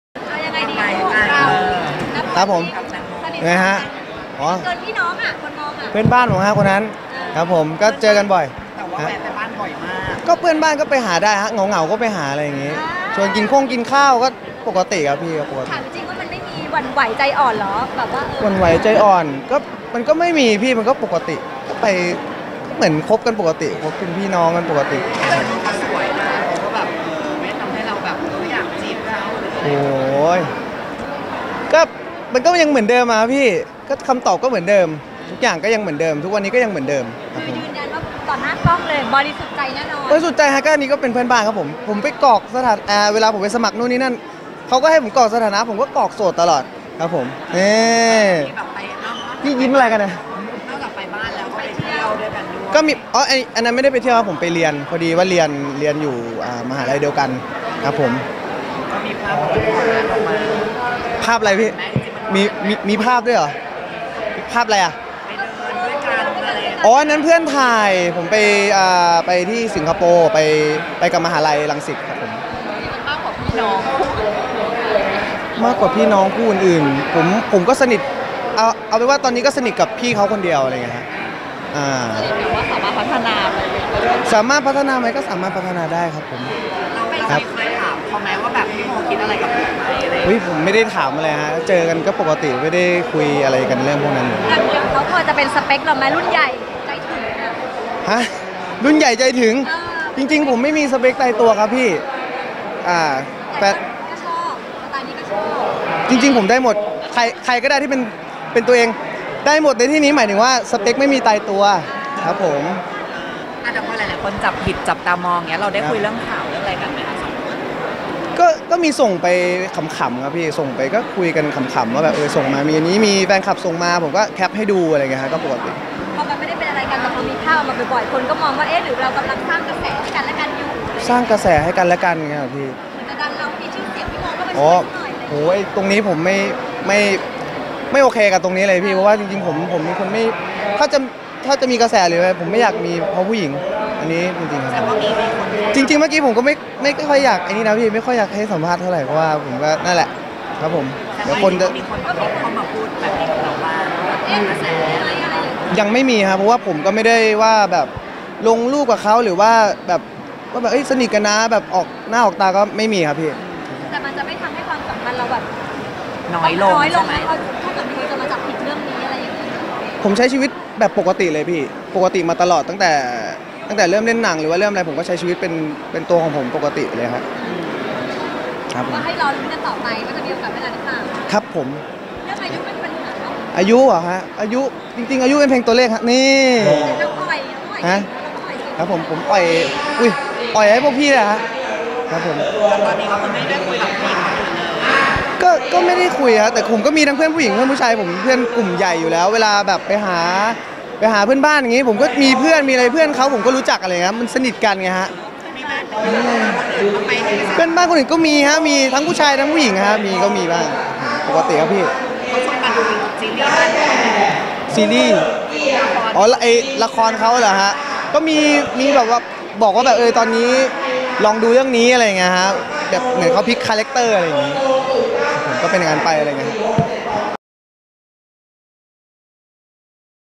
นิก คุณาธิป เผยสถานะกับ แตงโม นิดา พัฒนาได้ ลั่น! ตอนนี้คือพี่น้อง
นิก คุณาธิป ยันสถานะ กับ แตงโม นิดา คือพี่น้องกัน ที่อยู่ด้วยกันบ่อยเพราะบ้านใกล้กัน ย้ำคำตอบเหมือนเดิม รับเป็นคนที่สนิทอยู่คนเดียวและสามารถพัฒนาได้ ส่วนตัวไม่มีสเปค แจงภาพที่ไปสิงคโปร์ด้วยกันคือไปเรียน ไม่ใช่ไปเที่ยว ไม่ใช่ภาพหวานรูปนั้นเป็นรูปที่เพื่อนถ่ายให้ ลั่นไม่โอเคคนมองเป็นข่าวกันเพราะเกาะกระแสอีกฝ่าย บอกไม่อยากมีกระแสเพราะเรื่องผู้หญิง